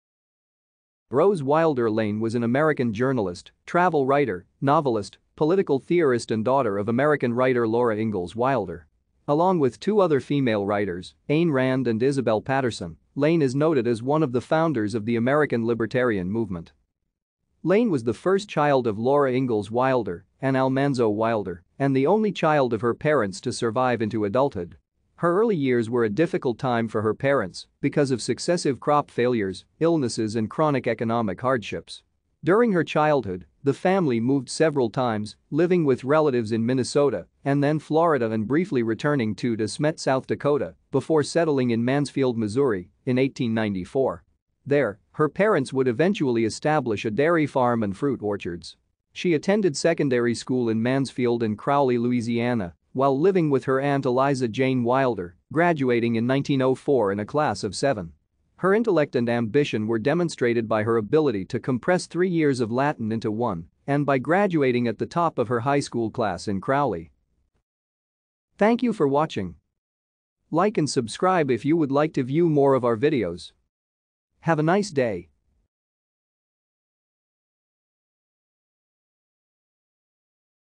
Rose Wilder Lane
Rose Wilder Lane  was an American journalist, travel writer, novelist, political theorist and daughter of American writer Laura Ingalls Wilder. Along with two other female writers, Ayn Rand and Isabel Paterson, Lane is noted as one of the founders of the American libertarian movement.